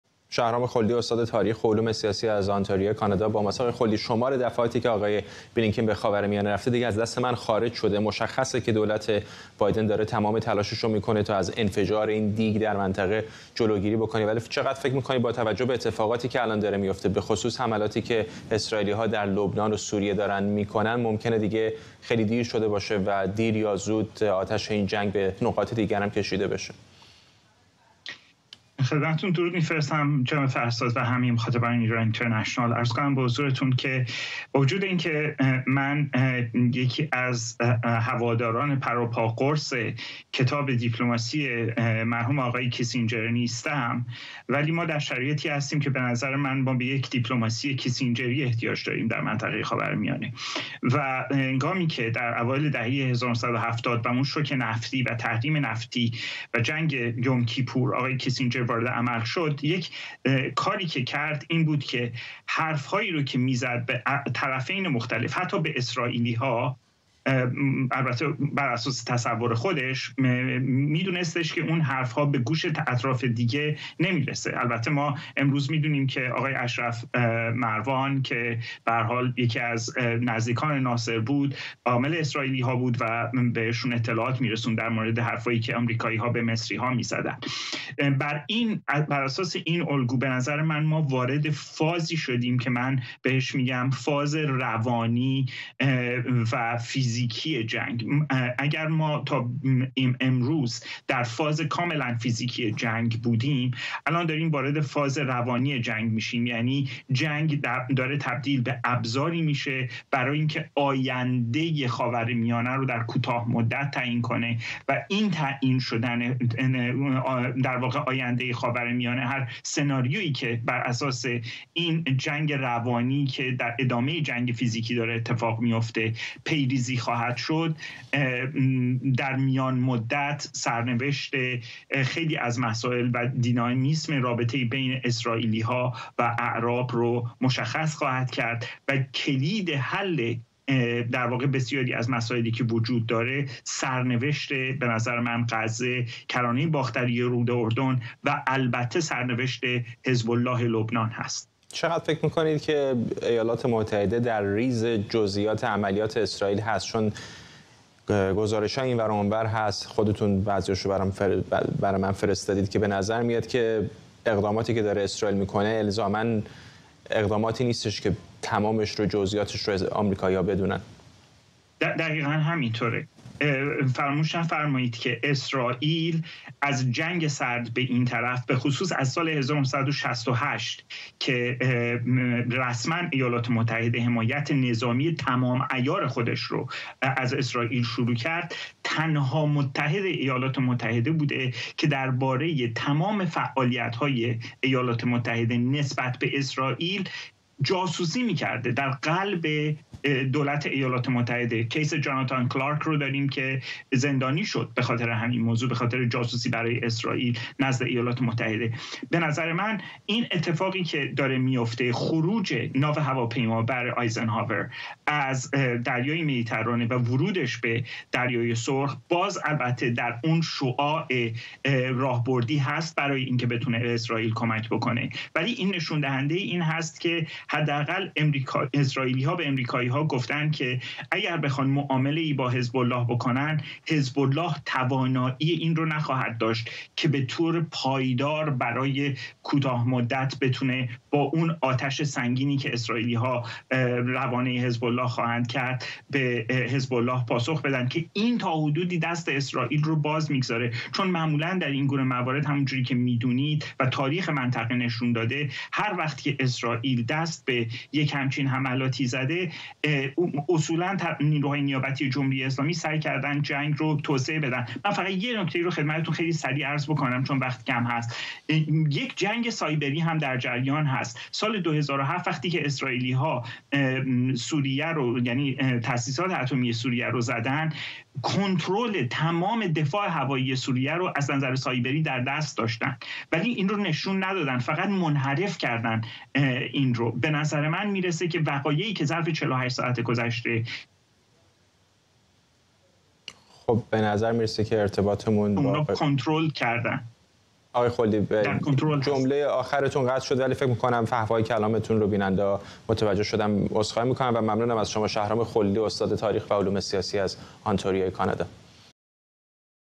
دیدار آنتونی بلینکن، وزیر خارجه آمریکا با سران امارت متحده عربی و عربستان سعودی
گفت‌وگو با شهرام خلدی، استاد تاریخ و علوم سیاسی